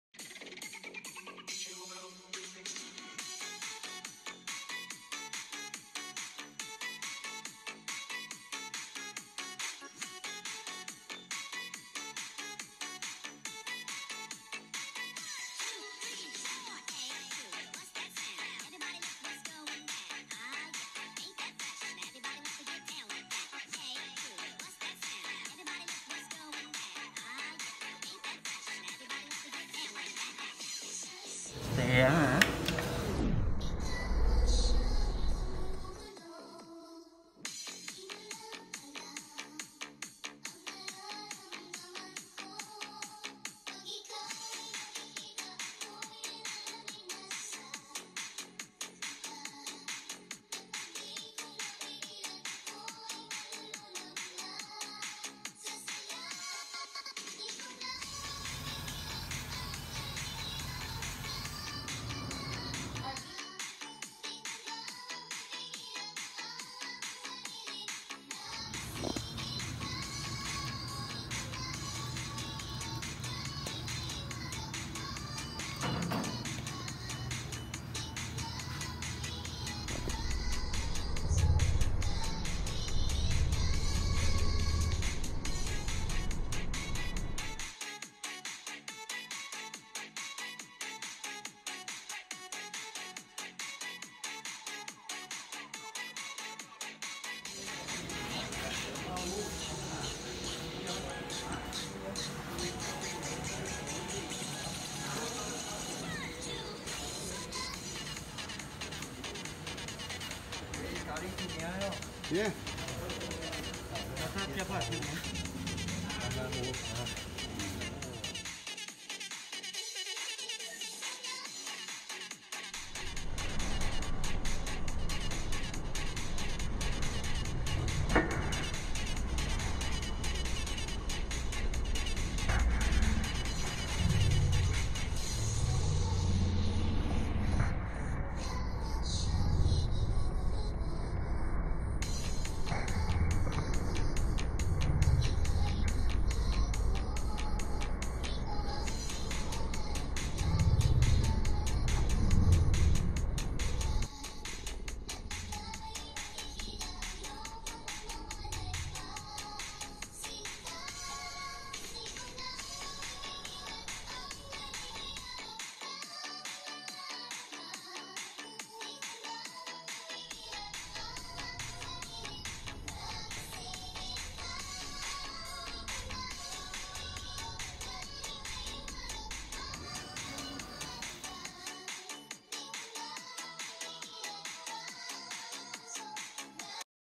Best Abalones,Sea Cucumber with Mushrooms
New City Restaurant in BM cook the Best Abalones, Sea Cucumber with Shitake Mushrooms , Braised Pig Legs is also very tasty, must Try